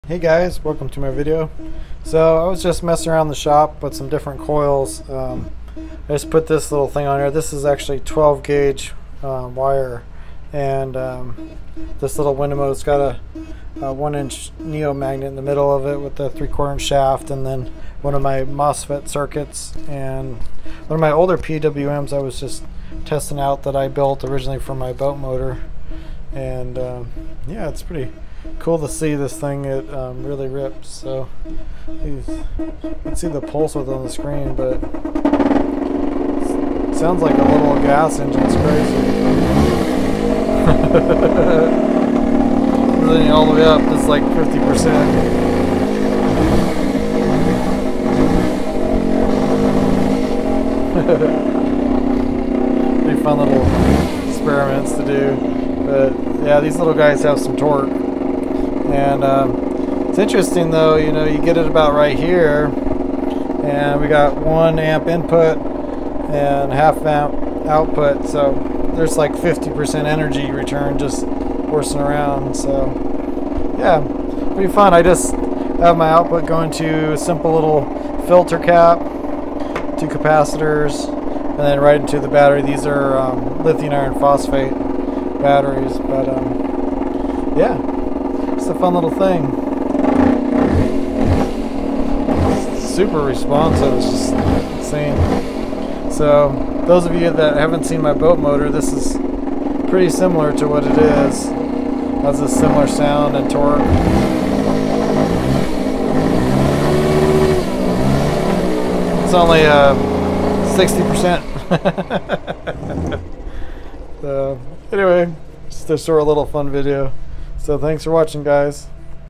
Window motor fun
Simple window motor with opto mosfet circuit and pwm !